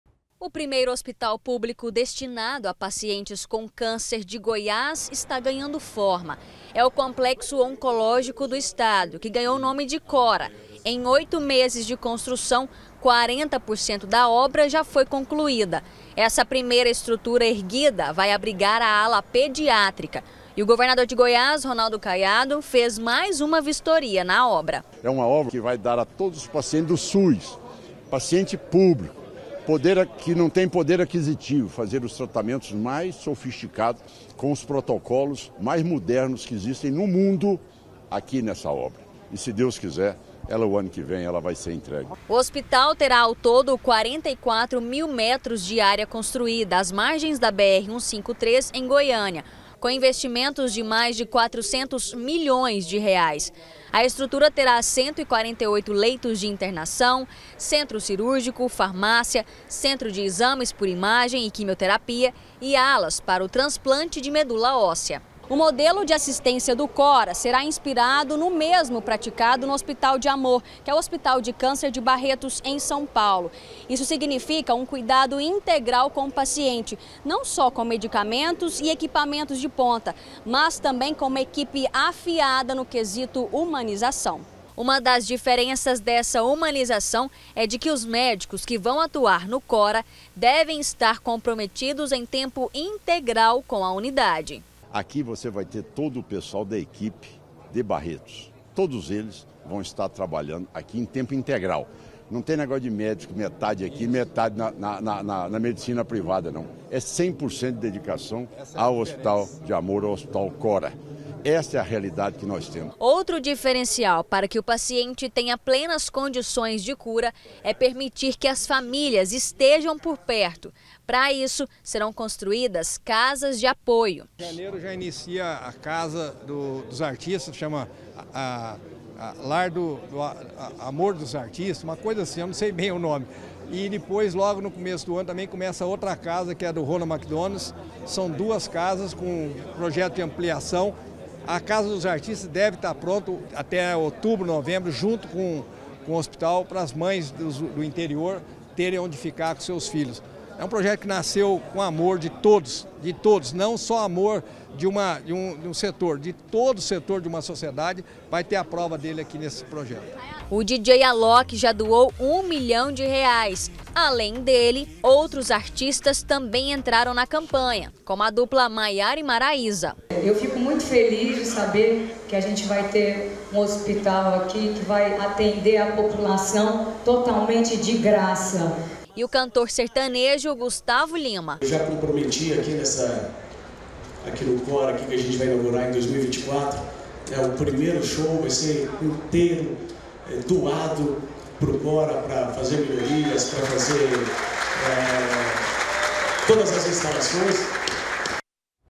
COMPLEXO ONCOLÓGICO: ALA PEDIÁTRICA SERÁ A PRIMEIRA A SER ENTREGUE
QUASE METADE DA CONSTRUÇÃO DO HOSPITAL DO CÂNCER  DE GOIÁS O CORA JÁ FOI CONCLUÍDA. O GOVERNADO RONALDO CAIADO ESTEVE HOJE NO CANTEIRO DE OBRAS.A ALA PEDIÁTRICA DEVE  SER A PRIMEIRA A SER ENTREGUE. A DUPLA MAIARA E MARAÍSA E O CANTOR GUSTAVO LIMA ESTIVERAM LA E SE COMPROMETERAM A AJUDAR.